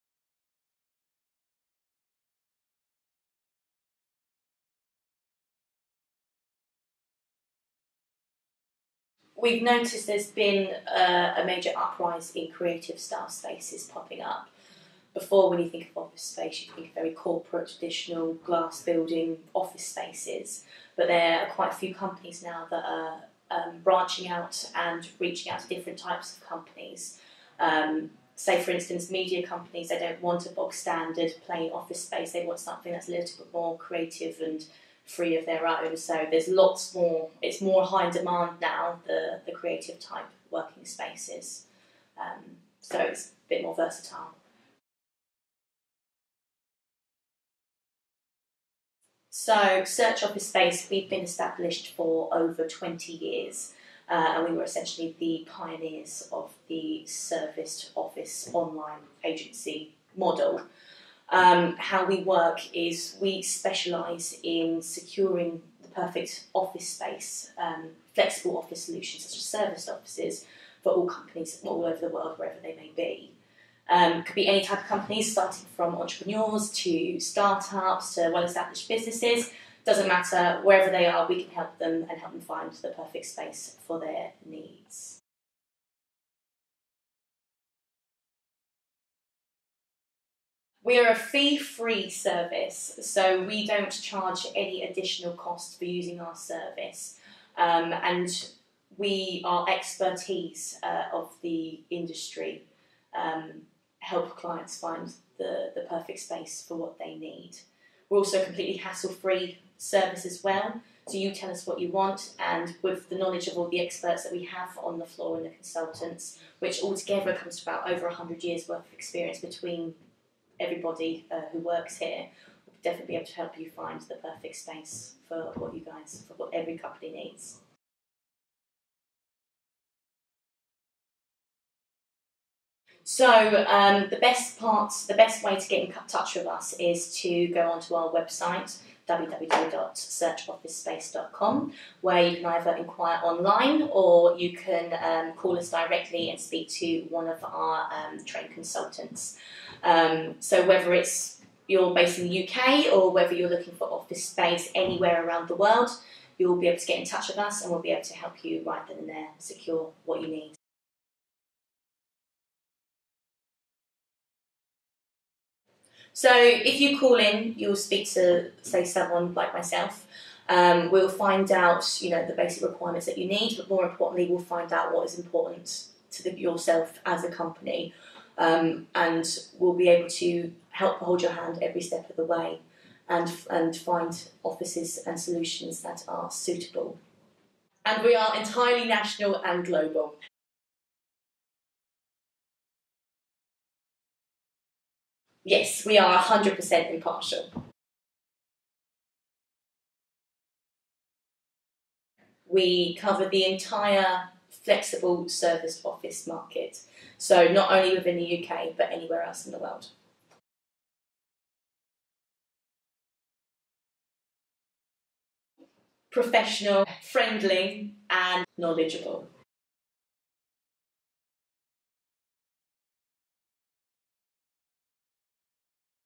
Why use Search Office Space?
Take a look into the Search Office Space HQ and see some of our team at work.  We will also explain why we are the company for you to use in finding the best workspace for you